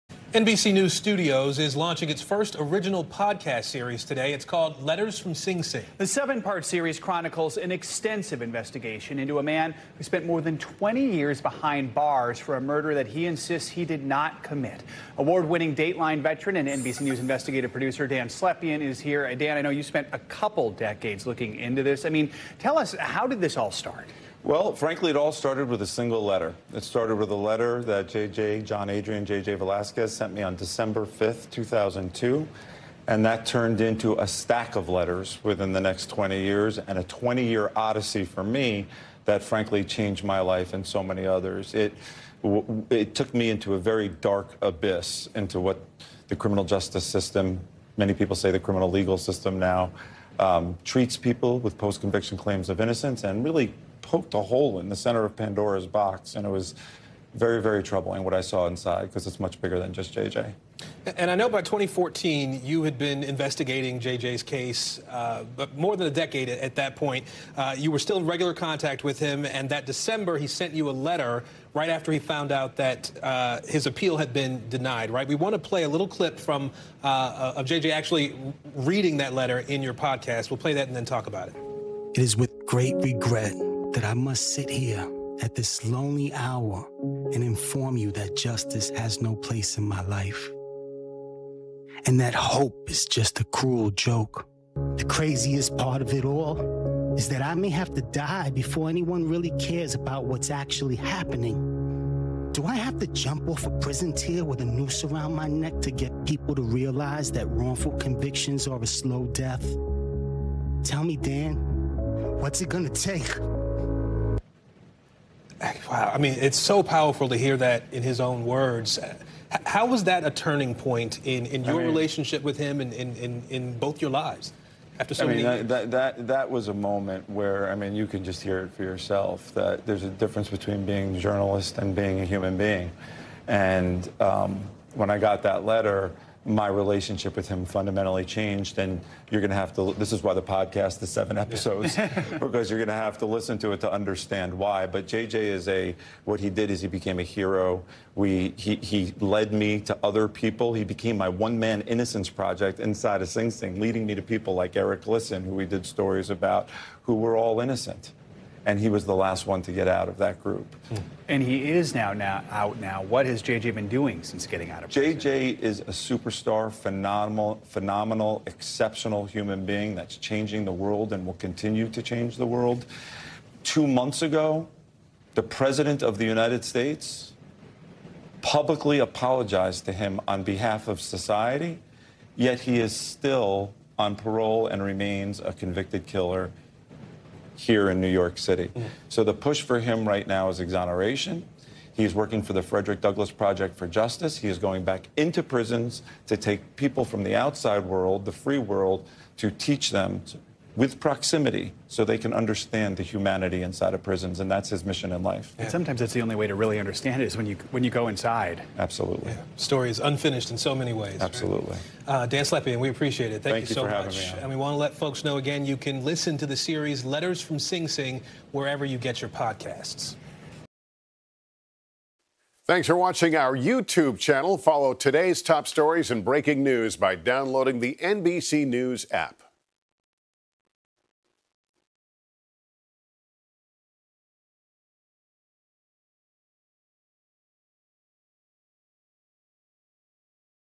‘Letters from Sing Sing’: NBC News podcast investigates high-profile murder case
NBC News Studios is launching its first original podcast series, titled “Letters from Sing Sing,” that follows an extensive investigation into a man who spent more than 20 years behind bars for a murder that he says he didn’t commit. NBC News investigative reporter Dan Slepian explains how the investigation began over 20 years ago.